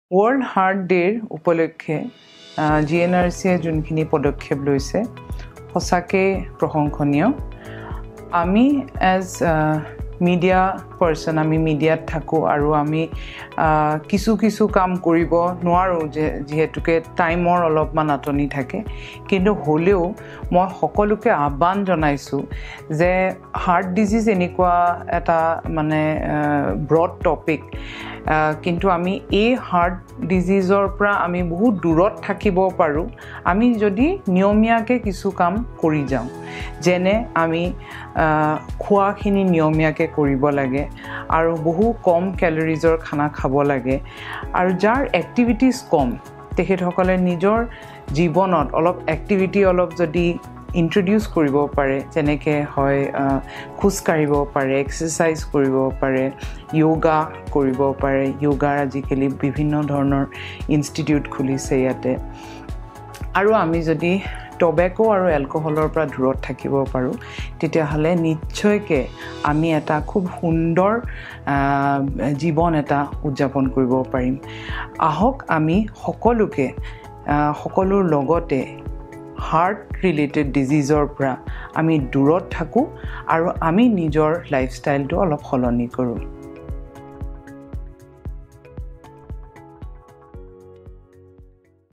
World Heart Day 2024 | Ms. Smitakshi B Goswami | GNRC Hospitals
In this video, Ms. Smitakshi B Goswami, Managing Director, Pratidin Time shares her thoughts on heart health and raising awareness about cardiovascular issues. Filmed on the occasion of World Heart Day, Ms. Smitakshi emphasizes the need for proactive health measures and lifestyle changes to promote heart health for all.

Join us in spreading awareness and learning from Ms. Smitakshi's insightful words. Let's work together to make heart health a priority!